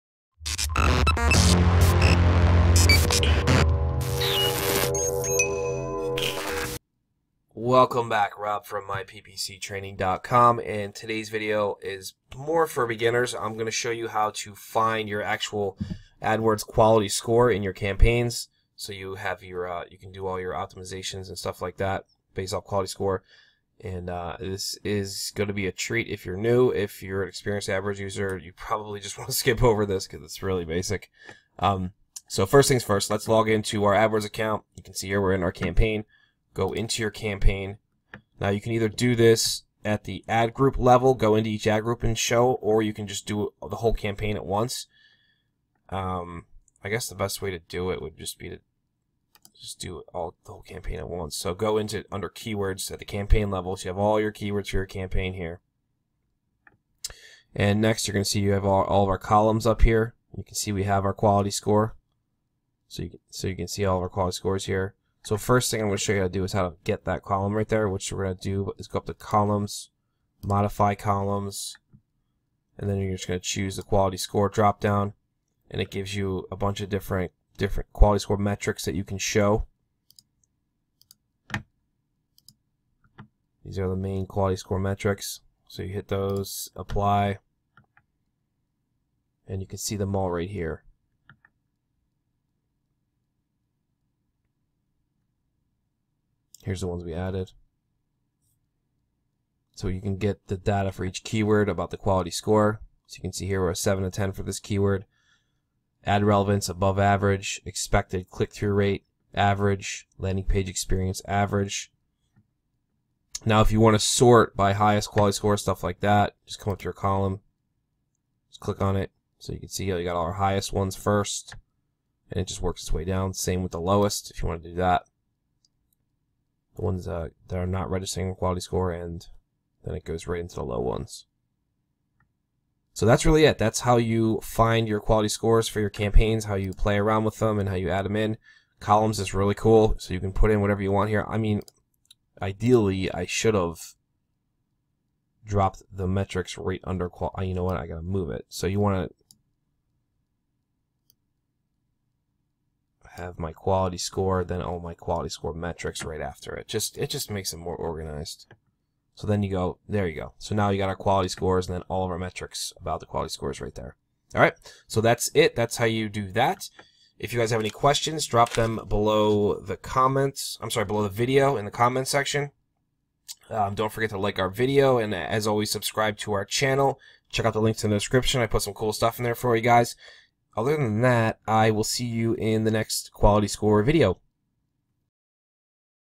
Quality Score In Adwords 💲 How Do I Find My Quality Score in AdWords?
Quality Score In Adwords 💲 How Do I Find My Quality Score in AdWords? In this Google Adwords tutorial we show you how to find and check your quality score in Adwords and how to increase and improve your quality score in Adwords. Adwords quality score is an extremely important number and will effect how much you pay per click and how well your Google ads campaign performs. Quality score is very important.

Adwords Ad Schedule HACK - 💰💰 Up To 70% Increase In Lead Quality With These 2 Simple Tweaks